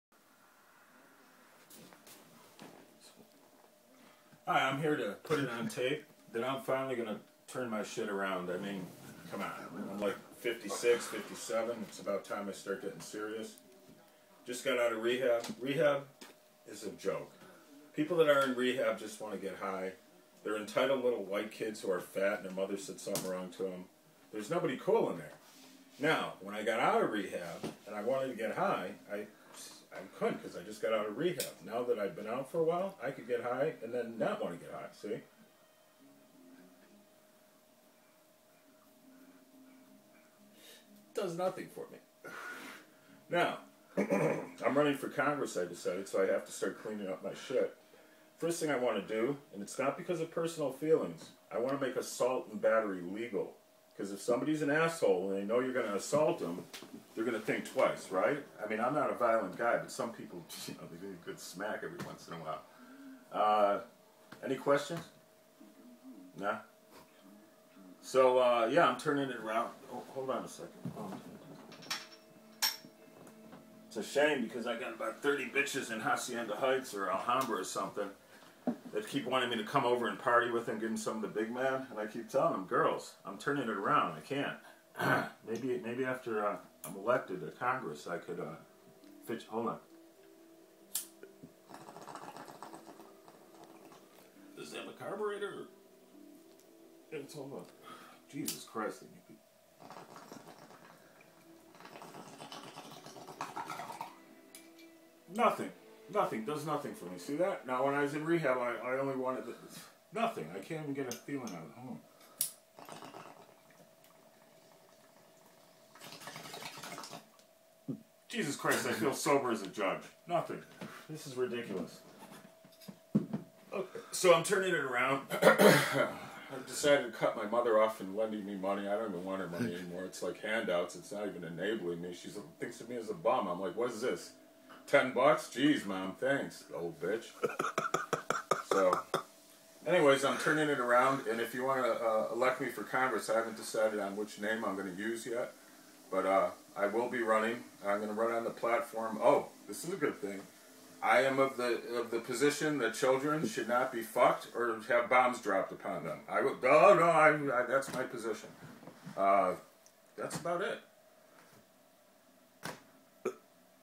Winning
Future Congressman Willie Williams Jr speaks about his run for office !